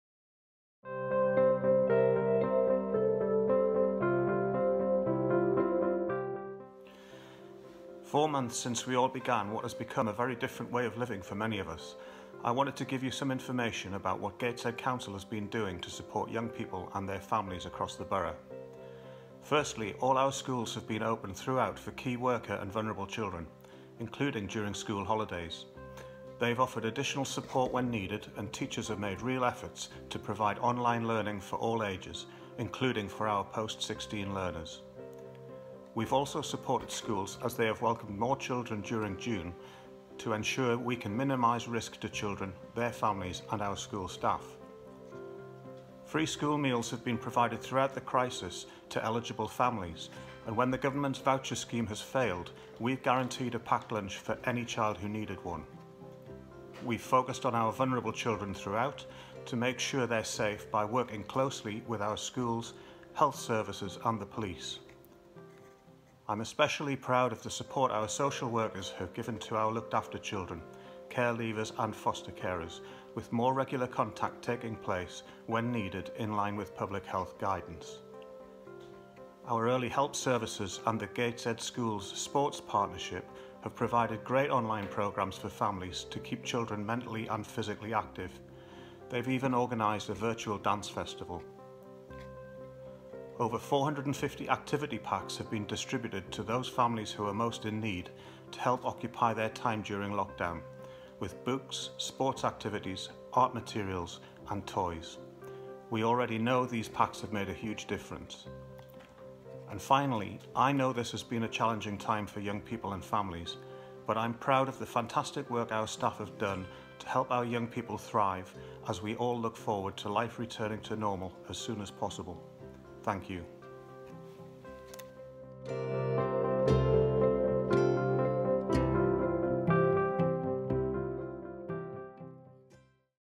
Cllr Gary Haley
How we have supported our young people and their families during the coronavirus pandemic